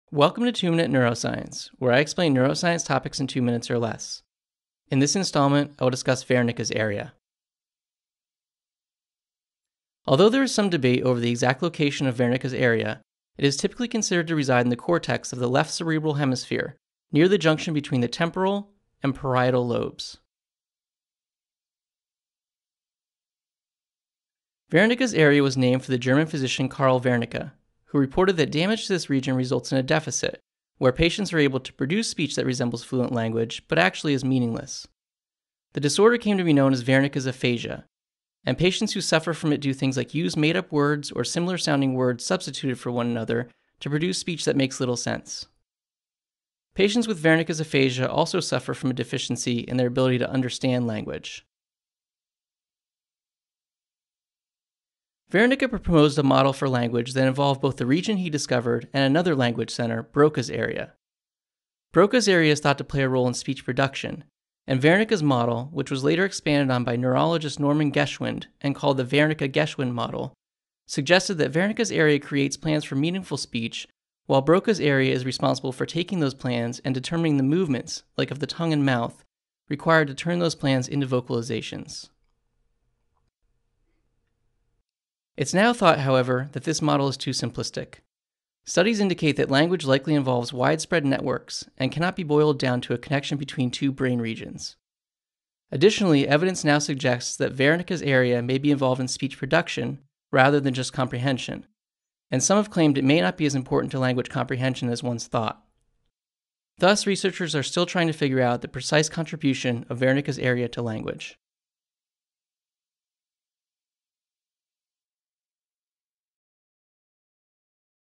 2-Minute Neuroscience: Wernicke's Area
Wernicke's area is a region in the cerebral cortex that historically has been considered important to language comprehension and the production of meaningful speech. In this video, I discuss the location of Wernicke's area, the deficit that occurs when it is damaged (Wernicke's aphasia), and hypotheses about its role in language.

For an article (on my website) that explains Wernicke's area, click this

TRANSCRIPT:

Welcome to 2 minute neuroscience, where I explain neuroscience topics in 2 minutes or less. In this installment I will discuss Wernicke’s area.

Although there is some debate over the exact location of Wernicke’s area, it is typically considered to reside in the cortex of the left cerebral hemisphere near the junction between the temporal and parietal lobes.

Wernicke’s area was named for the German physician Carl Wernicke, who reported that damage to this region results in a deficit where patients are able to produce speech that resembles fluent language but actually is meaningless. The disorder came to be known as Wernicke’s aphasia, and patients who suffer from it do things like use made-up words or similar-sounding words substituted for one another to produce speech that makes little sense. Patients with Wernicke’s aphasia also suffer from a deficiency in their ability to understand language.

Wernicke proposed a model for language that involved both the region he discovered and another language center: Broca’s area. Broca’s area is thought to play a role in speech production, and Wernicke’s model, which was later expanded on by neurologist Norman Geschwind and called the Wernicke-Geschwind model, suggested that Wernicke’s area creates plans for meaningful speech while Broca’s area is responsible for taking those plans and determining the movements (like of the tongue and mouth) required to turn those plans into vocalizations.

It’s now thought, however, that this model is too simplistic. Studies indicate that language likely involves widespread networks and cannot be boiled down to a connection between two brain regions. Additionally, evidence now suggests that Wernicke’s area may be involved in speech production rather than just comprehension, and some have claimed it may not be as important to language comprehension as once thought. Thus, researchers are still trying to figure out the precise contribution of Wernicke’s area to language.

REFERENCES:

Binder, JR. The Wernicke area: Modern evidence and a reinterpretation. Neurology. 2015; 85(24): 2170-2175.

Breedlove SM, Watson NV. Biological Psychology. 7th ed. Sunderland, MA: Sinauer Associates, Inc.; 2013.